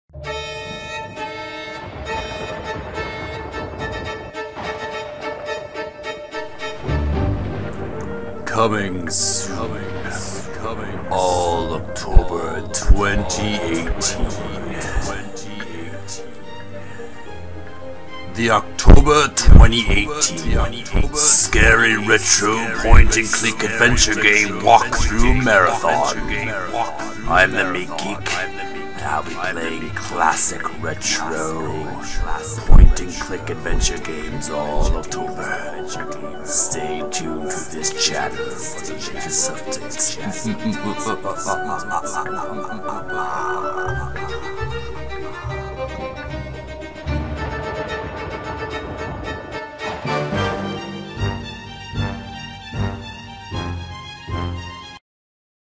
October 2018 - Scary Retro Point & Click Adventure Game Walkthrough Marathon - Intro - 2018-10-01
ALL October, The Meek Geek will be playing scary retro Point & Click Adventure Games!